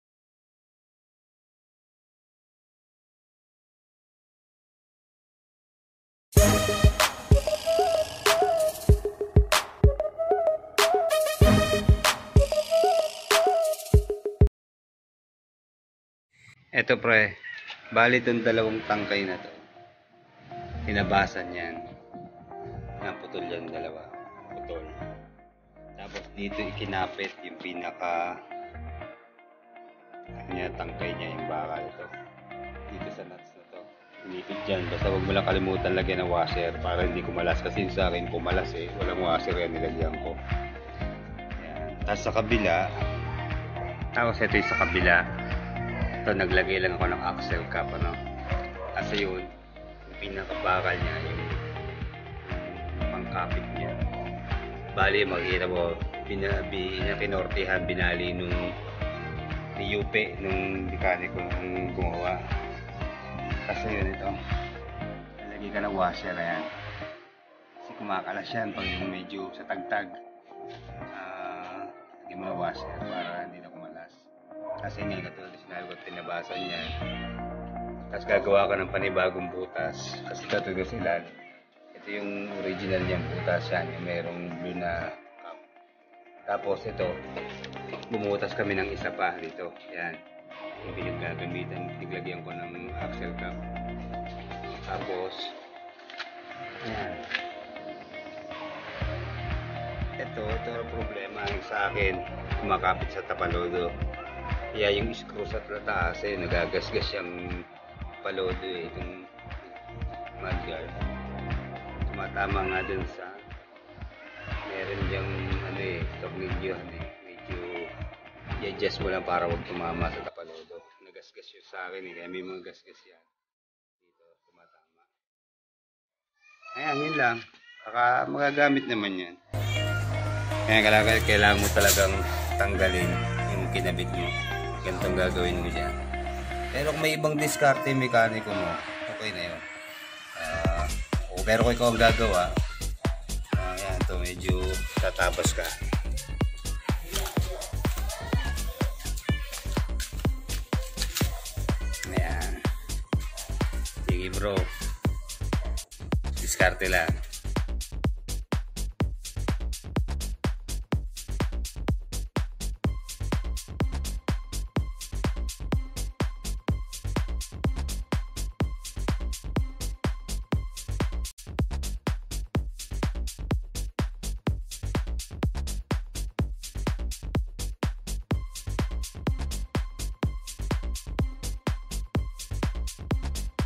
Rusi RFI 175 mudguard installation
Motorcycles MudGuard Universal foldability
A very effective shield on mud and dirt when riding a motorcycle,no more worries on your backride suffering from splash of rainwater,muds or dirt,with this mudguard it will lessen your stress on cleaning your motorcycle in a very cheap price you can avail it on shopee or lazada.see attached link below,thanks...

Rusi RFI 175cc
cash price: P90,000
Installment basis
downpayment: P10,000
monthly installment for 3 yrs: P3,420
(if payment is up to date)

Engine type (gy6)
SOHC,single cylinder,aircooled,fuel injected,
automatic transmission,4 stroke,2 valves,electric and kick starter,rear and front disc brake,equipped with combi brake system (CBS).

with telescopic fork front suspension and twin rear shock,headlights,tail lights and signal lights are all LED,with smart keyless technology,combined analog and digital.panel.

A big tank fuel capacity of 9.5 liters,with gas consumption of 2.6 liters in every 100kms ride,
And it has a very comfortable camel back type of seat that will never slip and will give us a nice riding position.